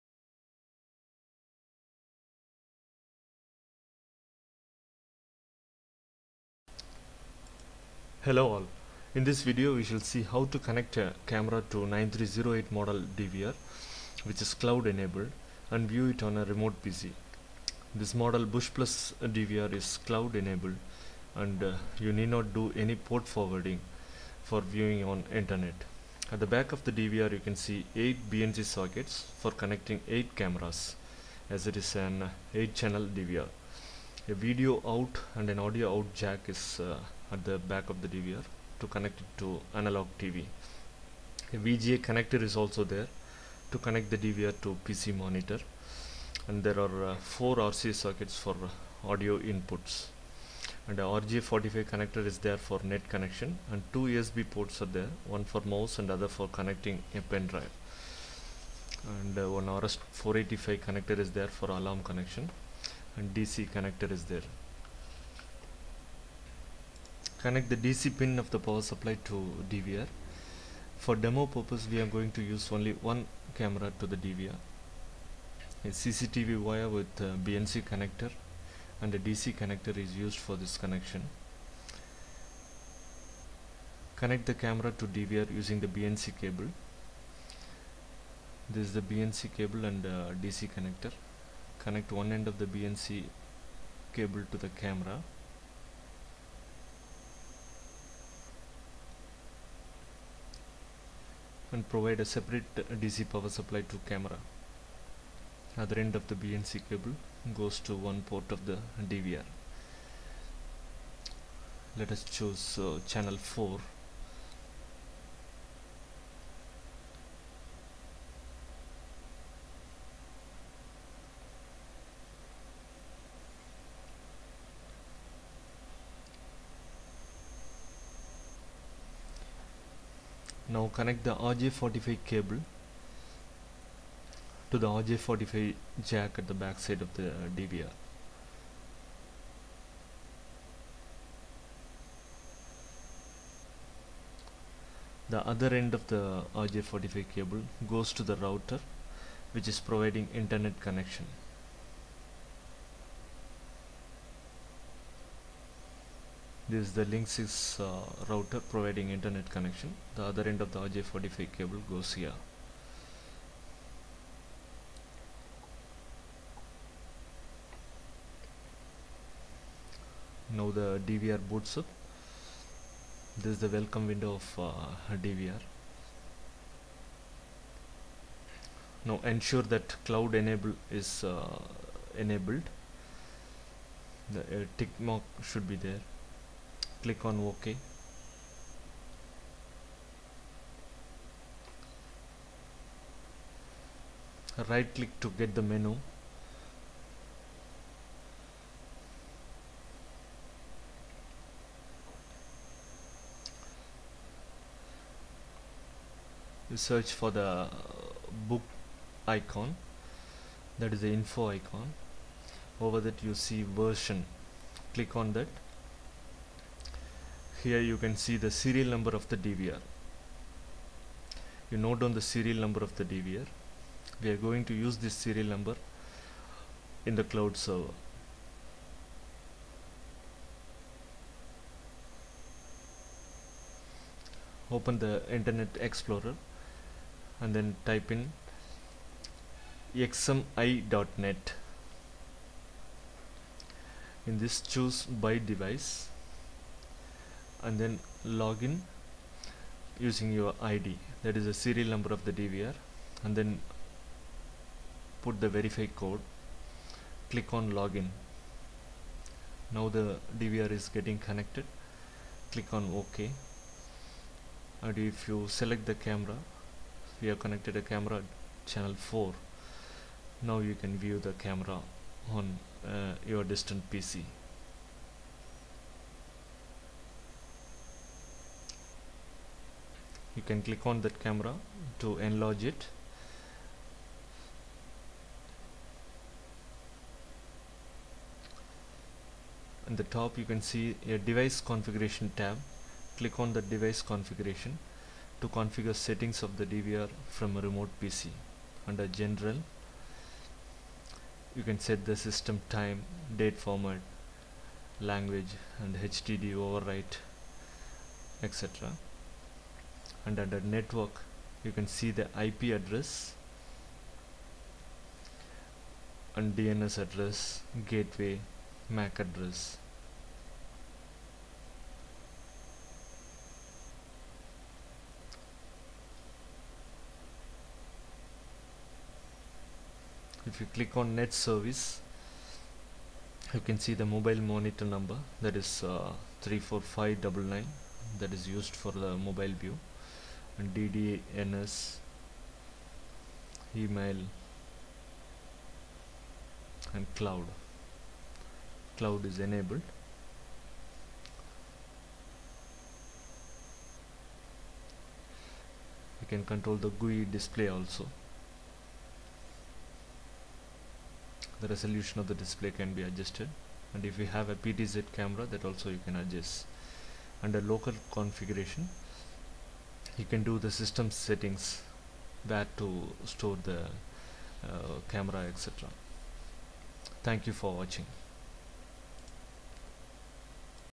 DVR - CLOUD ENABLED - No Port Forwarding to Remote View CCTV DVR
This video describes how to connect a camera to a cloud enabled DVR & control the Cameras connected to DVR from a Remote PC.
No Port Forwarding is needed in the Router setting . Just note the Serial number of DVR & feed it under the Device Info of